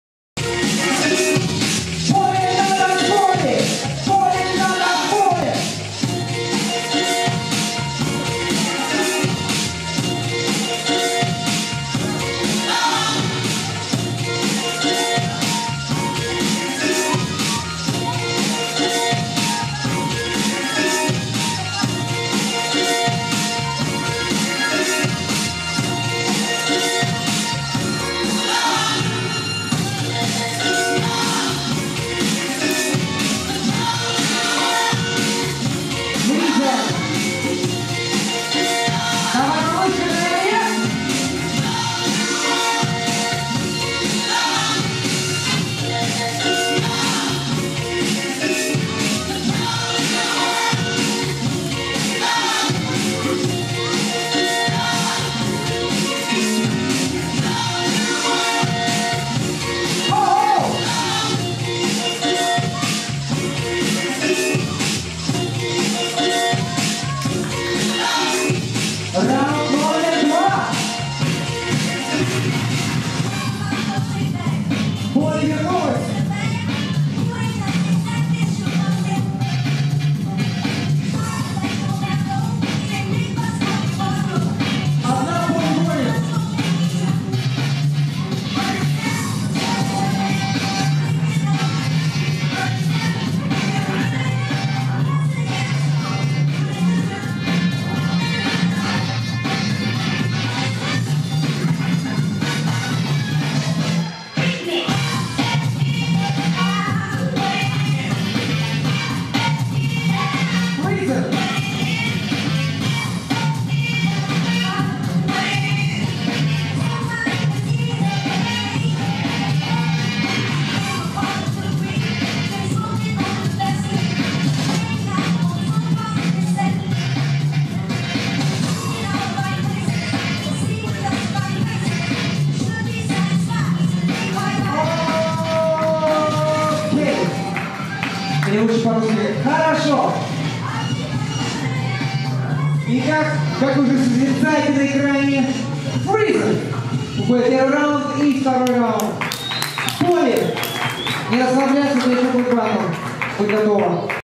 bgirls semifinal POLLY vs FreeZen - брейкданс чемпионат г.Химки
14 марта 2020 в г.Химки в Молодёжном центре "Восход" прошел брейкданс чемпионат и первенство по брейкингу.

Номинации: 7-10 лет, 11-13 лет, 14-15 лет, 16-18 лет, 19+